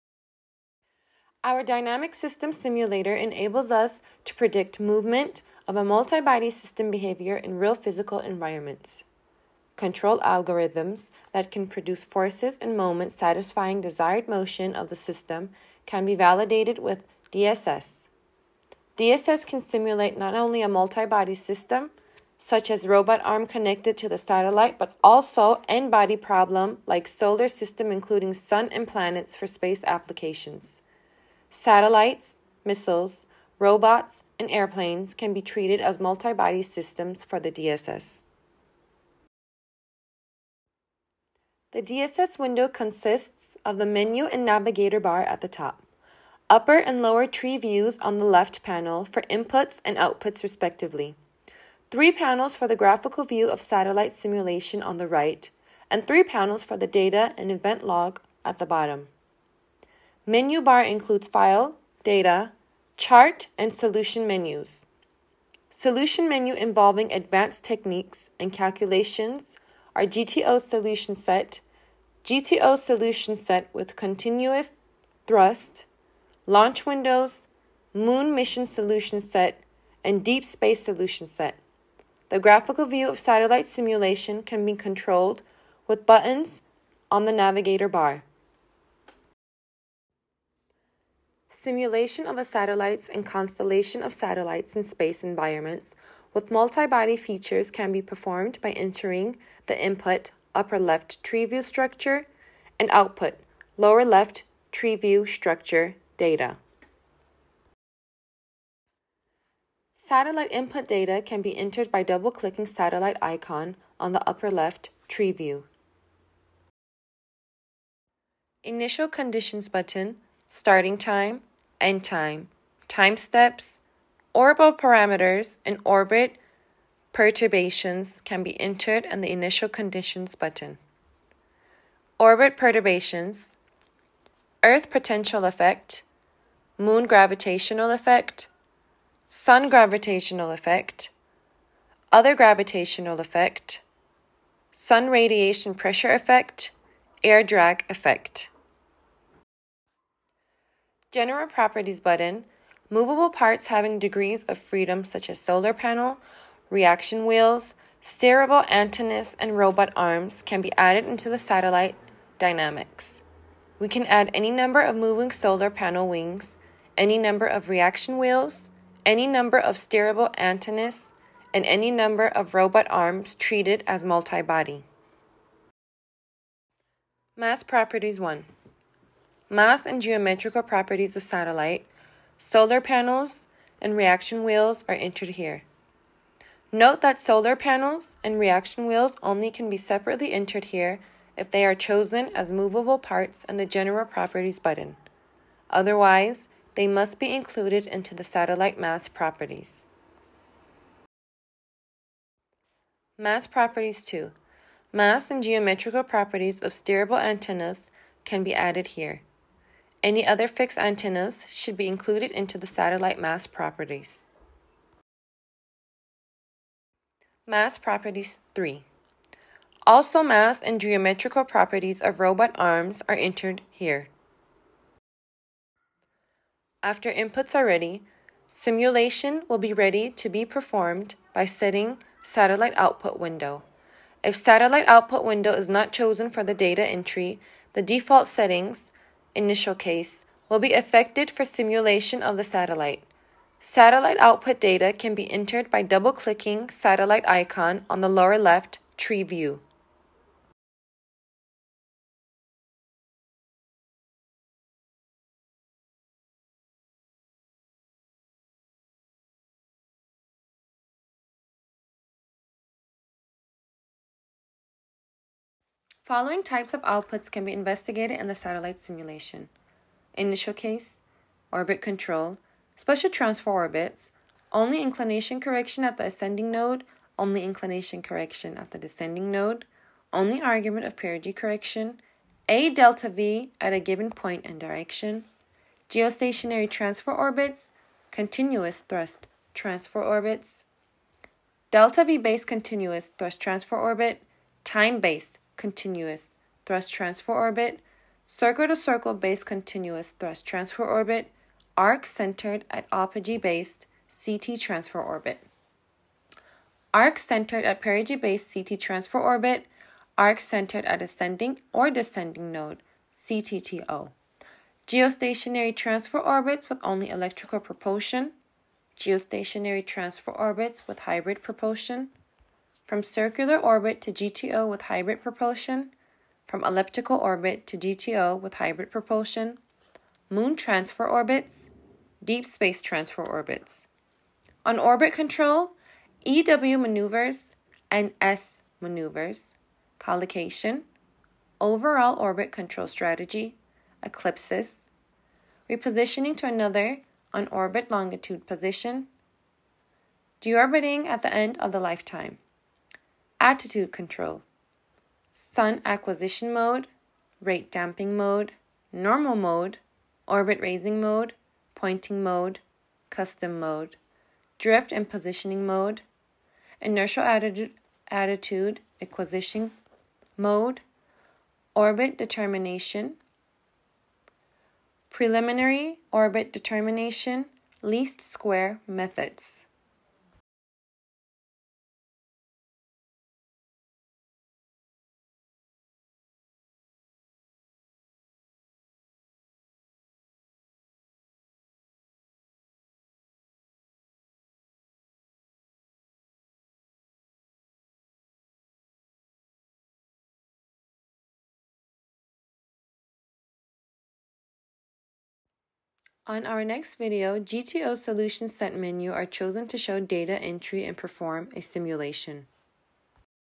Dynamic System Simulator | DSS
0:00 Dynamic System Simulator | DSS
0:41 Menu and Navigation
1:31 Entering the Input and Output
1:49 Input Data
1:58 Initial Conditions button
2:30 General Properties button
2:57 Mass Properties1
3:22 Mass Properties2
3:36 Mass Properties3
3:46 Output
4:25 Output Details

Our Dynamic System Simulator (DSS) enables us to predict movement of a multi-body system behavior in real-physical environments. Control algorithms that can produce forces and moments satisfying desired motion of the system can be validated with DSS. DSS can simulate not only a multi-body system such as robot arm connected to the satellite but also n-body problem like solar system including Sun and Planets for space applications. Satellites, missiles, robots, and airplanes can be treated as multi-body systems for the DSS.

The DSS window consists of the menu and navigator bar at the top, upper and lower TreeViews on the left panel for inputs and outputs respectively, three panels for the graphical view of satellite simulation on the right and three panels for the data and event log at the bottom. Menu bar includes File, Data, Chart and Solution menus. Solutions menu involving advanced technics and calculations are GTO Solution Set, GTO Solution Set with Continuous Thrust, Launch Windows, Moon Mission Solution Set and Deep Space Solution Set. The graphical view of satellite simulation can be controlled with buttons on the navigator bar.

Simulation of a satellites or constellation of satellites in space environments with multi-body features can be performed by entering the input (upper left TreeView structure) and output (lower left TreeView structure) data.

Satellite input data can be entered by double clicking satellite icon on the upper left TreeView.

Initial Conditions button: Starting time, end time, time steps, orbital parameters and orbit perturbations can be entered in the Initial Conditions button.

 Orbit perturbations:
  Earth Potential Effect
  Moon Gravitational Effect
  Sun Gravitational Effect
  Other Gravitational Effect
  Sun Radiation Pressure Effect
  Air Drag Effect

General Properties button: Movable parts having degrees of freedom such as Solar Panel, Reaction Wheels, Steerable Antennas and Robot Arms can be added into the satellite dynamics. We can add any number of moving solar panel wings, any number of reaction wheels, any number of steerable antennas and any number of robot arms treated as multi-body.

Mass Properties1: Mass and geometrical properties of Satellite, Solar Panels, and Reaction Wheels are entered here. Note that Solar Panels, and Reaction Wheels only can be separately entered here if they are chosen as movable parts in the General Properties button. Otherwise they must be included into the satellite mass properties.

Mass Properties2: Mass and geometrical properties of Steerable Antennas can be added here. Any other fixed Antennas should be included into the satellite mass properties.

Mass Properties3: Also Mass and geometrical properties of Robot Arms are entered here.

After inputs are ready, Simulation will be ready to be performed by setting Satellite output window. If Satellite output window is not chosen for the data entry, the default settings (Initial Case) will be effected for simulation of the satellite. Satellite output data can be entered by double clicking satellite icon on the lower left TreeView.

Following types of outputs can be investigated in the satellites simulation:
 Initial Case
 Orbit Control
  Special Transfer Orbits
   Only inclination correction at the ascending node
   Only inclination correction at the descending node
   Only Argument of perigee correction
   A Delta V at a given point and direction
  Geostationary Transfer Orbits
  Continuous-Thrust Transfer Orbits
   Delta-V based Continuous-Thrust Transfer Orbit
   Time based Continuous-Thrust Transfer Orbit
   Circle to Circle based Continuous-Thrust Transfer Orbit
   Arc centered at apogee based CT Transfer Orbit
   Arc centered at perigee based CT Transfer Orbit
   Arc centered at Ascending or Descending node CTTO
  Geostationary Transfer Orbits with Only Electrical Propulsion
  Geostationary Transfer Orbits with Hybrid Propulsion
   From Circular Orbit to GTO with hybrid Propulsion
   From Elliptical Orbit to GTO with hybrid Propulsion
  Moon Transfer Orbits
  Deep Space Transfer Orbits
   On Orbit Control
   E/W Maneuvers
   N/S Maneuvers
   Collocation
   Overall Orbit Control Strategy
   Eclipses
   Repositioning to Another On-Orbit Longitude Position
   Deorbiting at the End of the Lifetime
  Attitude Control
    Sun Acquisition Mode
   Rate Damping Mode
   Normal Mode
   Orbit Raising Mode
   Pointing Mode
   Custom Mode
   Drift and Positioning Mode
   Inertial Attitude Acquisition Mode
  Orbit Determination
   Preliminary Orbit Determination
   Least Square Methods

On our next video, GTOSolutionSet menu are chosen to show data entry and perform a simulation.